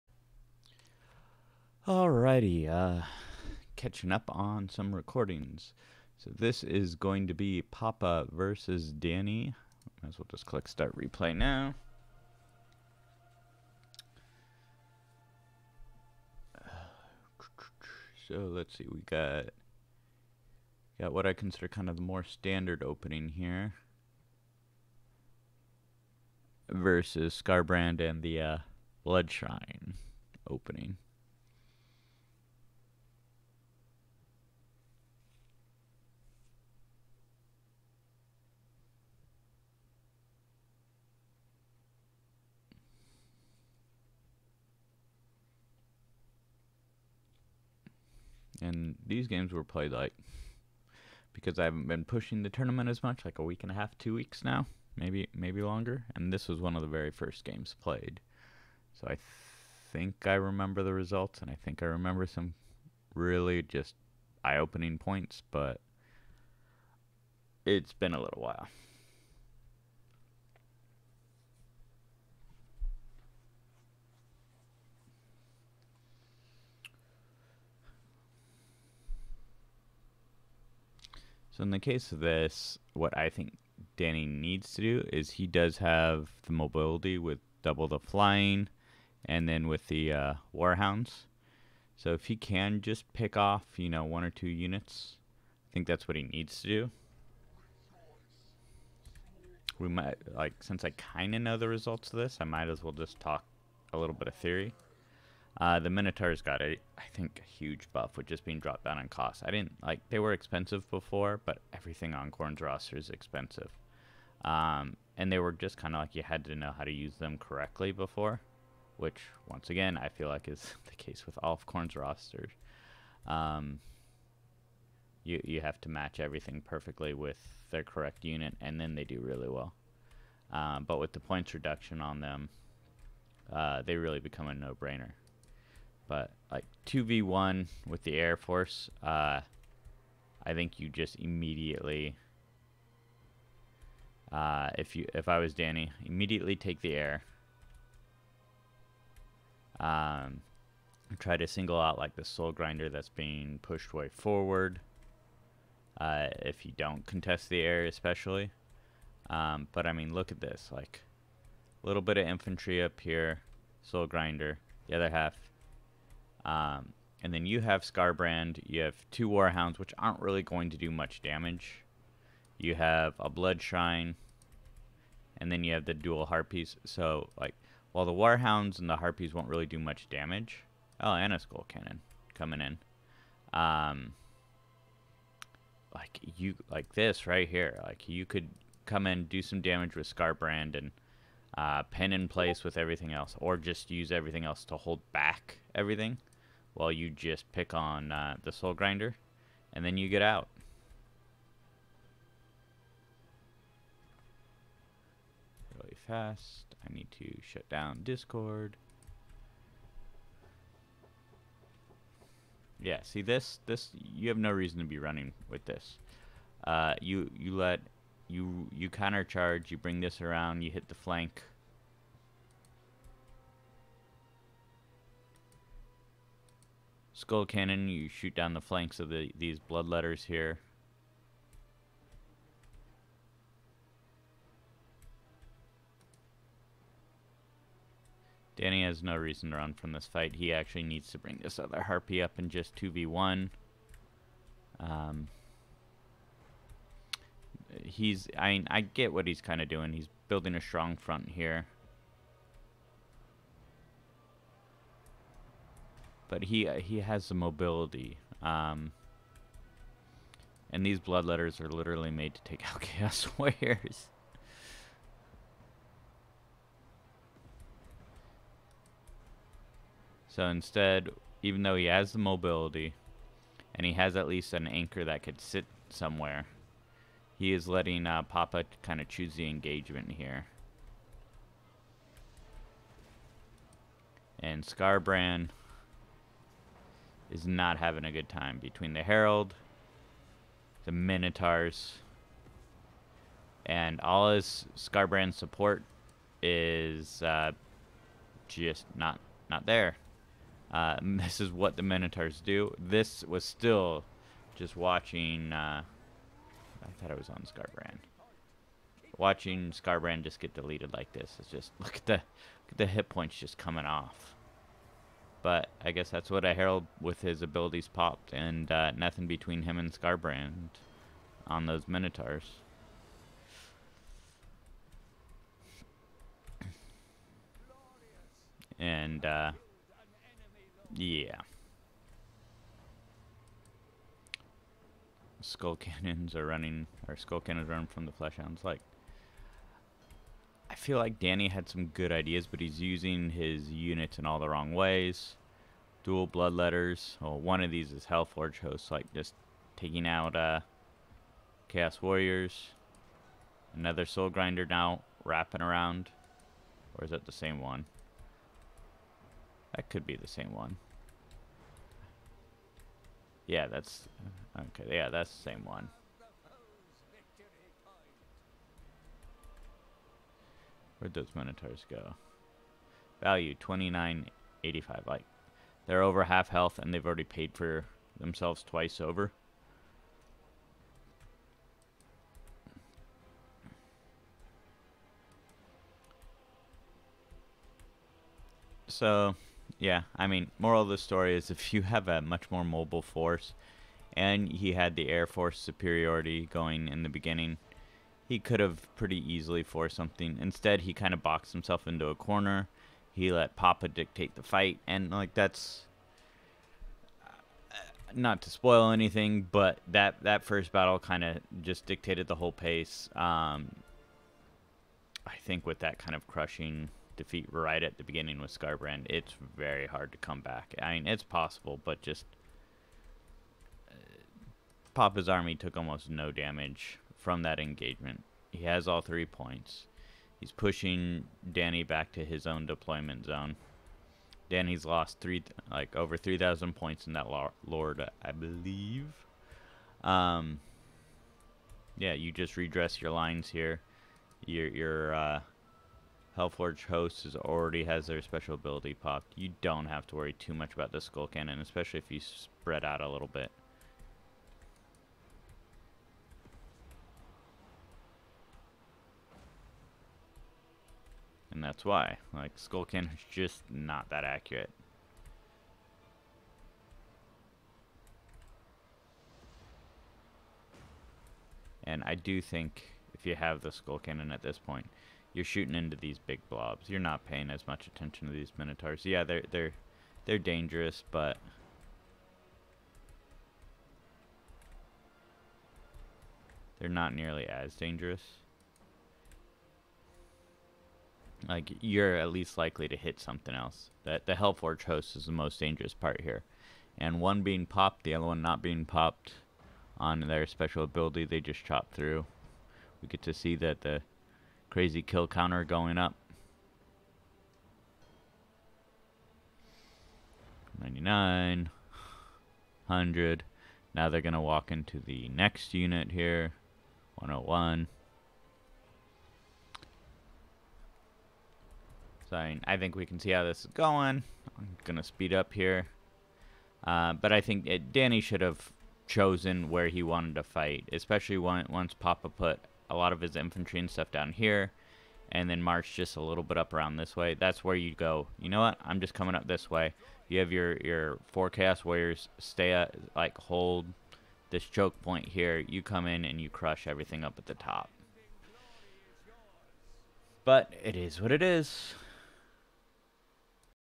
Gobbo Camp Skarbrand gets a couple new Scars
Gobbo Camp TT Tourney . Taking a swing at Khorne isn't a big deal for Skarbrand, but Fighting Minotaurs..Now that's scary